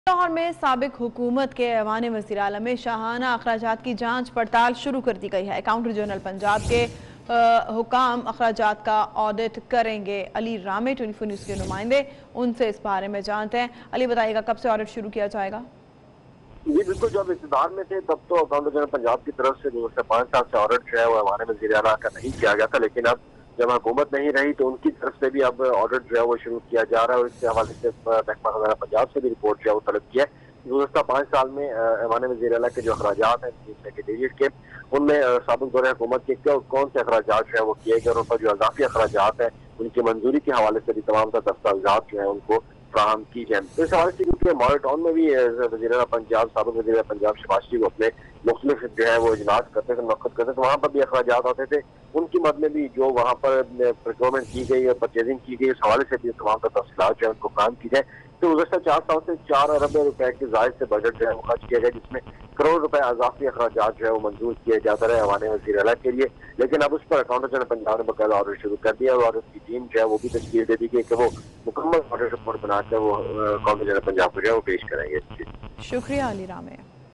Finance Department Punjab To Audit Luxuries Expenses Of Shahbaz Sharif | 24 News HD
24 News HD is one of the leading news channels of Pakistan bringing you the latest current affairs from Pakistan and around the world.